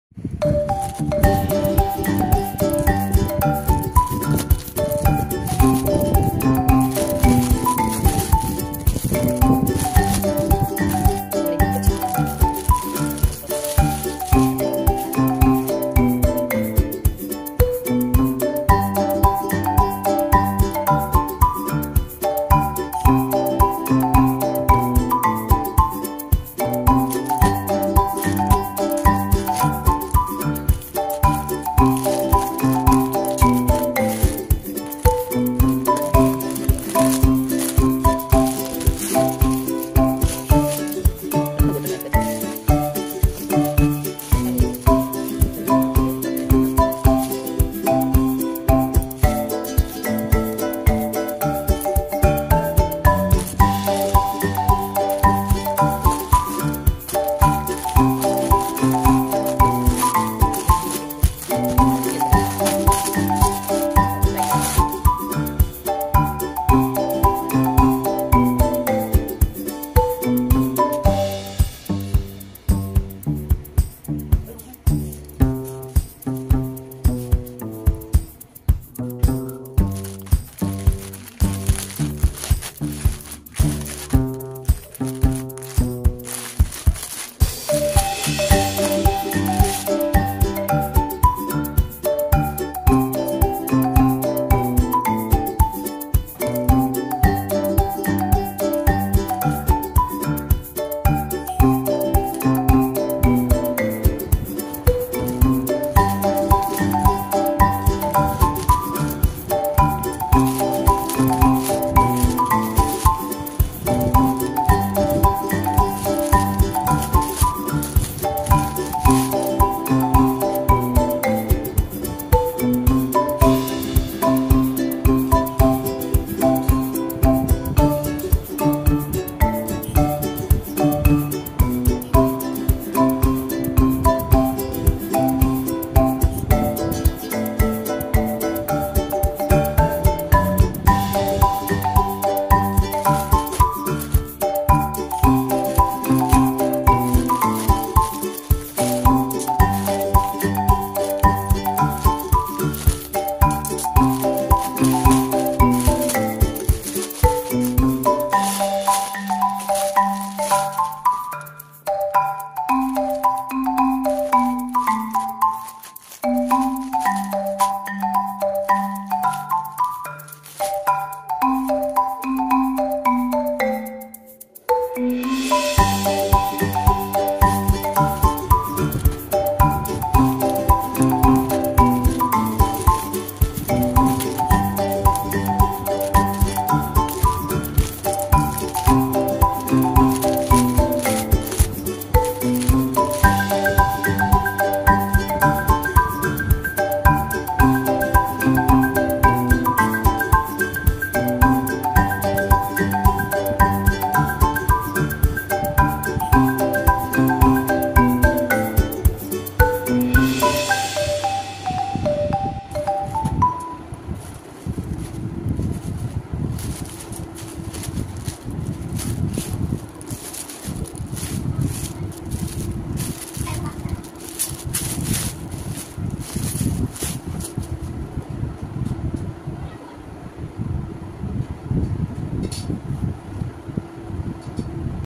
Meesho Saree Hull/Top Trending Saree Unboxing Video/Online Shopping Review With Shivani .
Instagram handal
shivanidas667